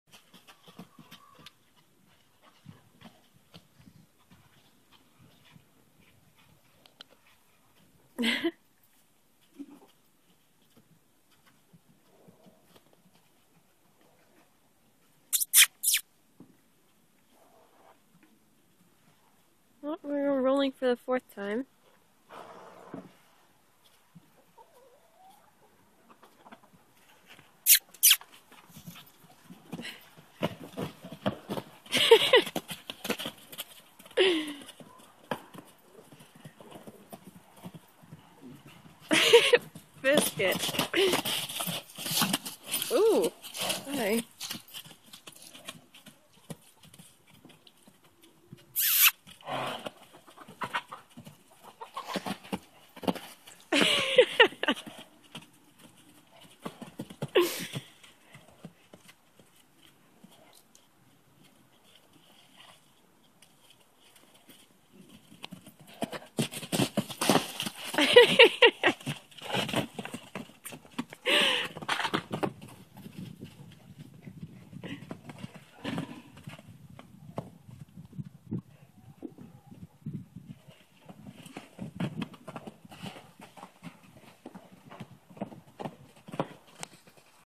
Energized
After not being turned out for a while the horses had lotsss of energy